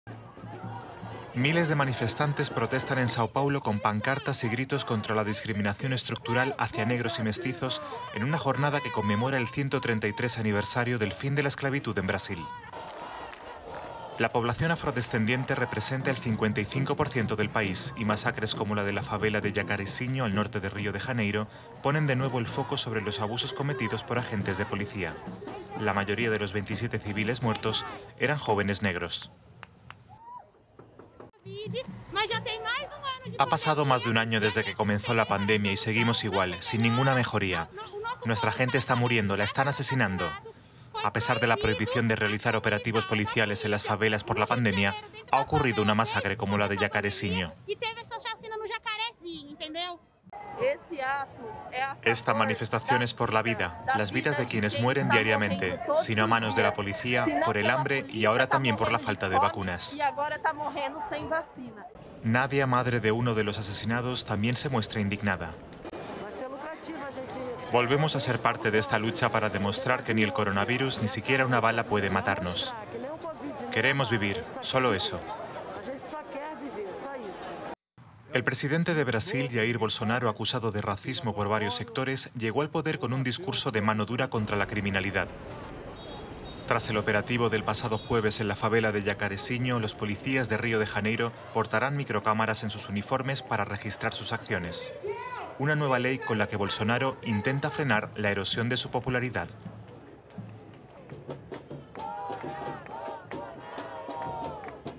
Brasil, a 133 años de la abolición de la esclavitud
Brasil conmemora el 133 aniversario del fin de la esclavitud con protestas dirigidas al gobierno de Jair Bolsonaro y contra el racismo estructural en un país con un 55 por ciento de población afrodescendiente